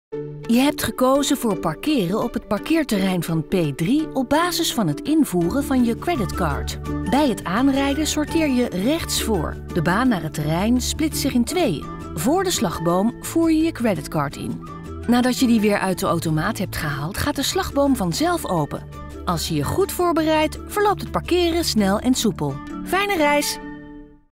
Zo parkeer je met je creditcard op het Schiphol P3 Parkeerterrein
Bij je reservering heb je gekozen om met creditcard in te rijden. Wij laten je in een paar stappen zien hoe je het parkeerterrein oprijdt door je creditcard in te voeren.